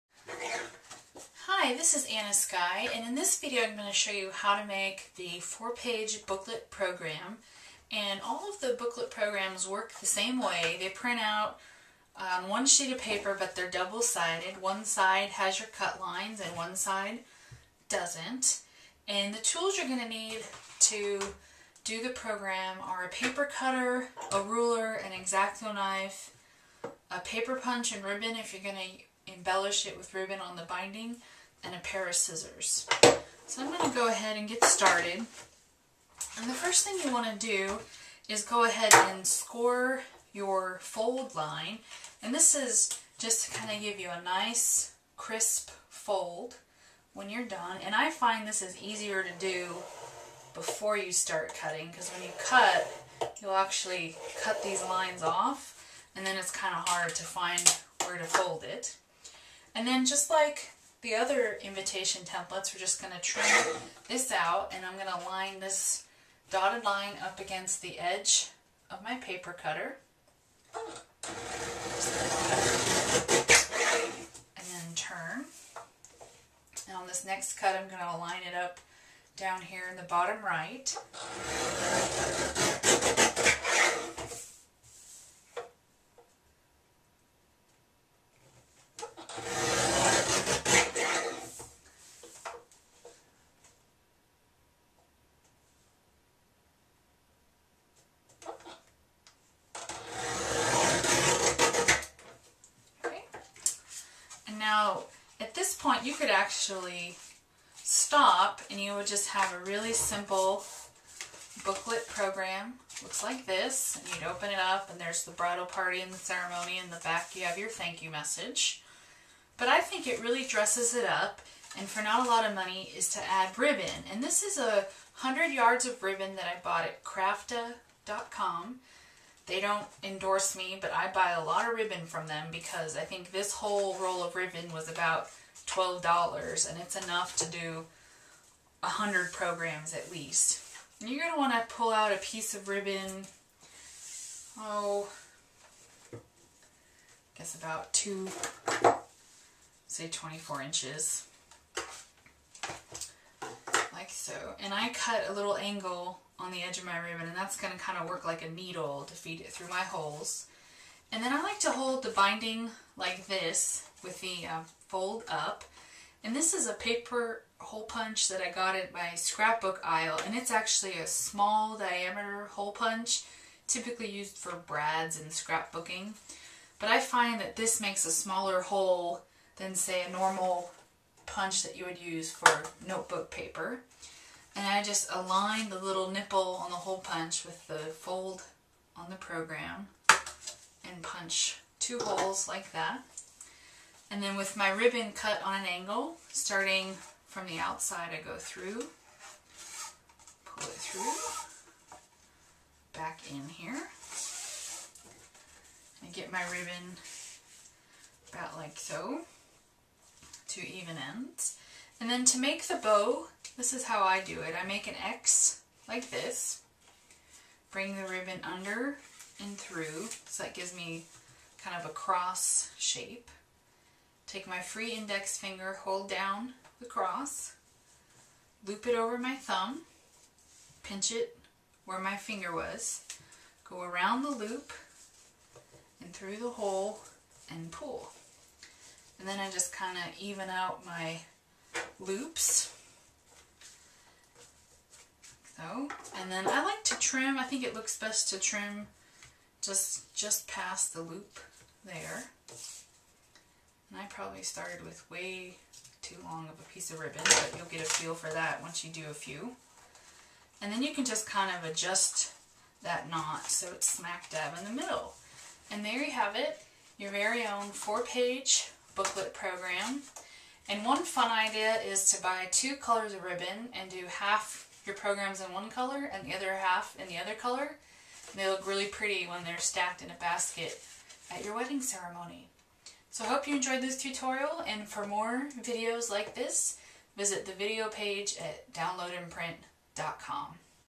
How to Make a Wedding Program
2019 Update: Now you can customize our designs directly online. Add your wording, change colors, and download a print-ready file.

Anna Skye shows you how to make a wedding program using the MS Word templates from Download & Print.